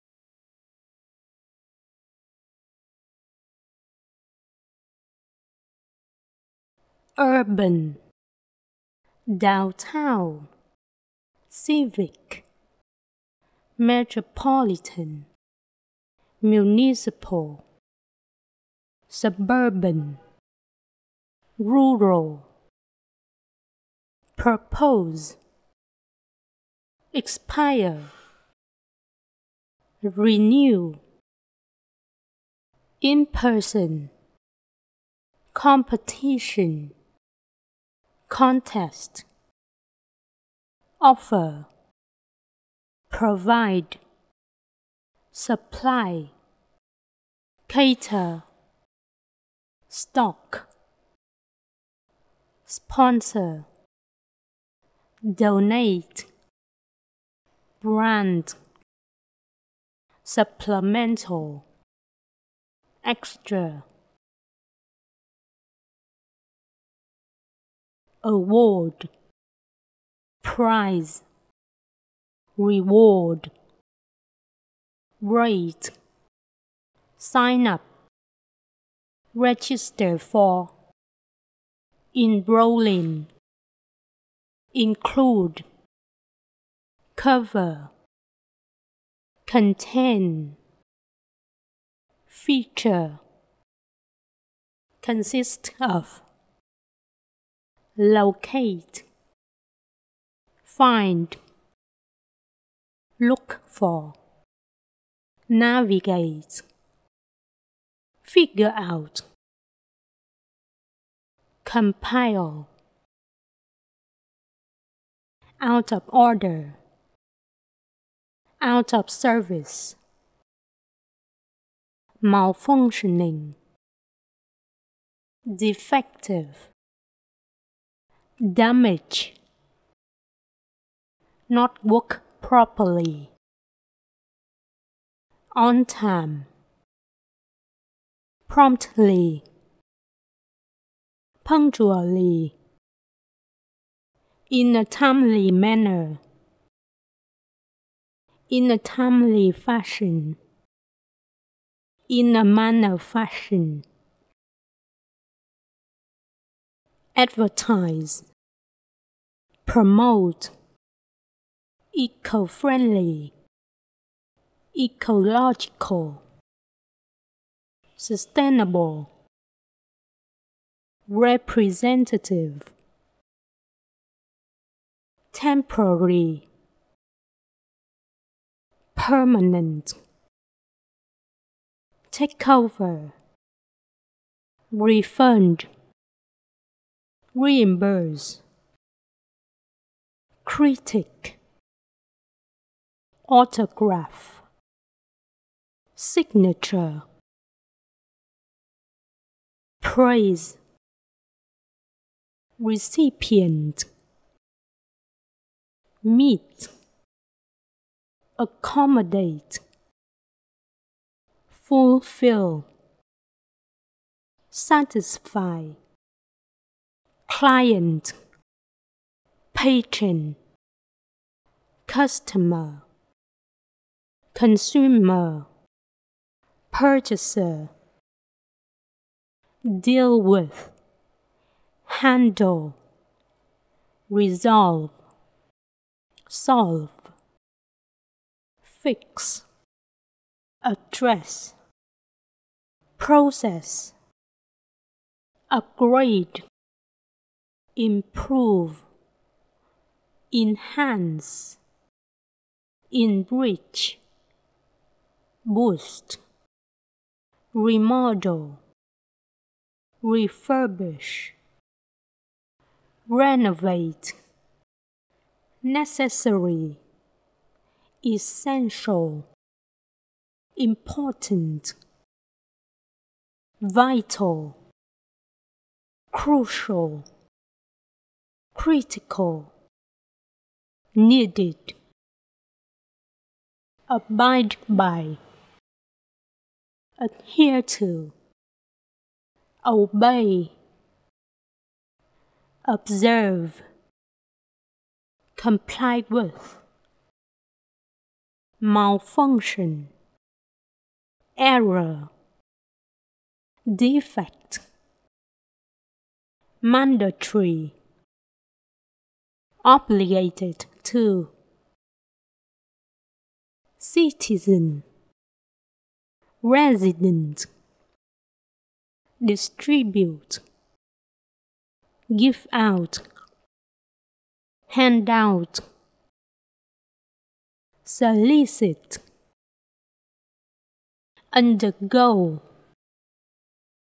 Tổng Hợp Từ Vựng Thường Xuyên Xuất Hiện Trong Part 7 Toeic Reading 2024
rural = suburban (adj) nông thôn
urban = downtown = civic = metropolitan = municipal (adj) thành phố
propose (v) đề xuất
expire (v)  hết hạn
renew (v)  gia hạn
in person - trực tiếp
competition = contest (n) cuộc thi
offer = provide = supply = cater = stock (v) cung cấp
supplemental = extra = additional (adj) bổ sung, phụ, thêm vào
award = prize = reward (n) phần thưởng
rate (n) giá cả, tỷ lệ
sign up = register for = enroll in (v) đăng ký
include = cover = contain = house = store = feature = consist of (v) bao gồm
locate = find = look for = navigate = figure out (v) xác định vị trí, tìm kiếm
compile (v) tập hợp, biên soạn
out of order = out of service = malfunctioning = defective = damage = not work properly – bị hư, trục trặc
on time = promptly = punctually = in a timely manner = in a timely fashion = in a manner fashion : đúng giờ
advertise = promote (v) quảng cáo
eco-friendly = ecological = sustainable (adj) thân thiện với môi trường
representative (n) người đại diện (tương tác với khách hàng)
temporary (adj) tạm thời
permanent (adj) cố định, lâu dài
take over (v) tiếp quản, đảm nhận
refund = reimburse (v) trả lại tiền
meet = accommodate = fulfill = satisfy (v) cung cấp, đáp ứng
critic (n) nhà phê bình
autograph = signature (n) chữ ký
praise (n) lời khen
recipient (n) người nhận
client = patron = customer = consumer = purchaser: khách hàng
deal with = handle = resolve = solve = fix = address = process (v) xử lý, giải quyết
upgrade = improve = enhance = promote = enrich = boost = remodel = refurbish = renovate
(v) nâng cấp, cải thiện
necessary = essential = important = vital = crucial = critical = needed (adj) cần thiết, chủ yếu
abide by = adhere to = obey = observe = comply with (v) tuân thủ
malfunction = error = defect (n) sự cố, trục trặc, bị lỗi
mandatory = obligated to (adj) bắt buộc
citizen = resident (n) người dân
distribute = give out = hand out (v) phân phối
solicit (v) xin (tiền, ý kiến)
undergo (v) trải qua